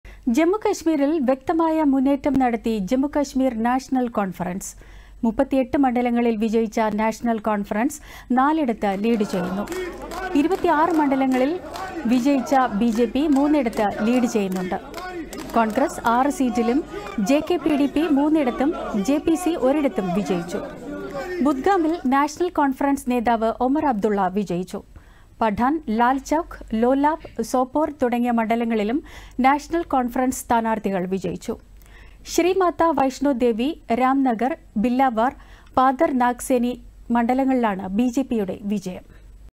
ജമ്മു കശ്മീരിൽ വ്യക്തമായ മുന്നേറ്റം നടത്തി ജമ്മു കശ്മീർ നാഷണൽ കോൺഫറൻസ്
38 മണ്ഡലങ്ങളിൽവിജയിച്ച നാഷണൽ കോൺഫറൻസ് നാലിടത്ത് ലീഡ് ചെയ്യുന്നു.

26 മണ്ഡലങ്ങളിൽ മണ്ഡലങ്ങളിൽ വിജയിച്ച ബിജെപി മൂന്നിടത്ത് ലീഡ് ചെയ്യുന്നുണ്ട്.

കോൺഗ്രസ് 6 സീറ്റിലും ജെ കെ പിഡിപി മൂന്നിടത്തും ജെപിസി ഒരിടത്തും വിജയിച്ചു.

ബുദ്ഗാമിൽ നാഷണൽ കോൺഫറൻസ് നേതാവ് ഒമർ അബ് ദുള്ള വിജയിച്ചു.

പഠാൻ , ലാൽചൌക്ക്, ലോലാബ്, സോപോർ തുടങ്ങിയ മണ്ഡലങ്ങളിലും നാഷണൽ കോൺഫറൻസ് സ്ഥാനാർത്ഥികൾ വിജയിച്ചു.

ശ്രീമാതാ വൈഷ്ണോദേവി, രാംനഗർ, ബില്ലാവാർ, പാതർ-നാഗ് സെനി മണ്ഡലങ്ങളിലാണ് ബിജെപിയുടെ വിജയം.